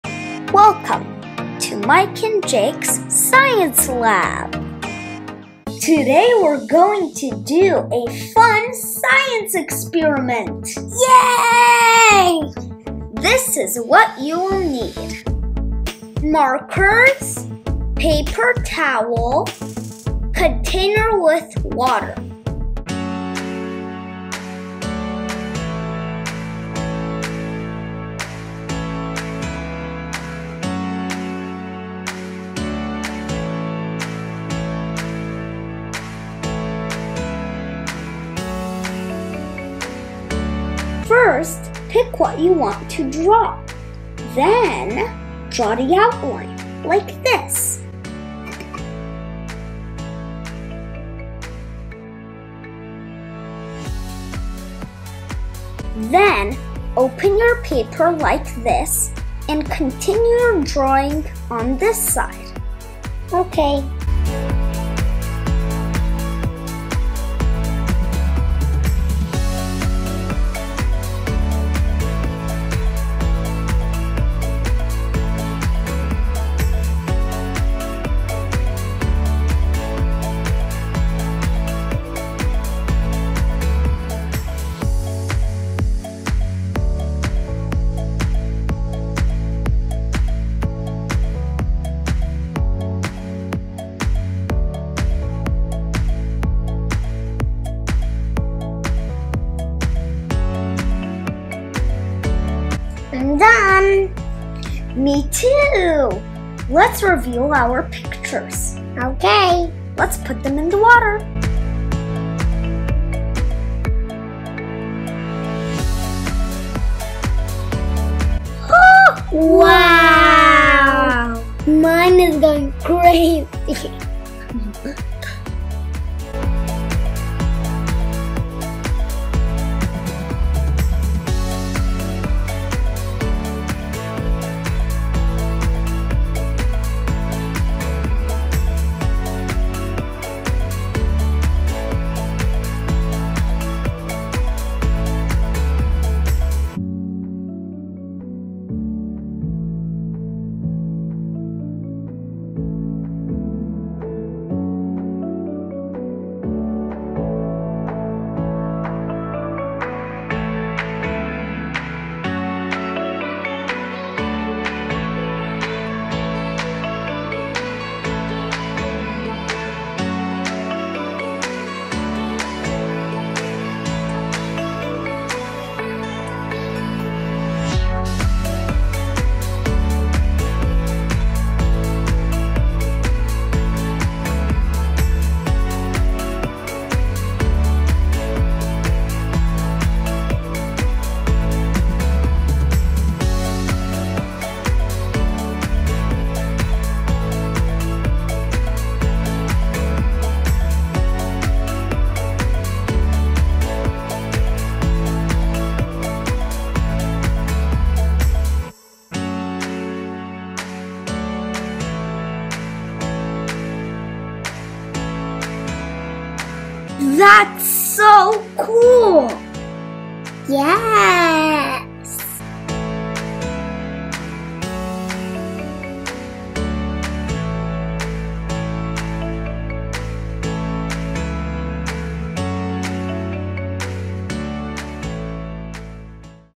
Magic Paper Towel Art Easy DIY Science Experiment for kids | Experiment with Mike and Jake प्रयोग
Paper towel magic trick DIY is fun and easy science experiment for kids to do at home.
Watch the pictures come to life in front of your eyes.

Educational science experiment for children.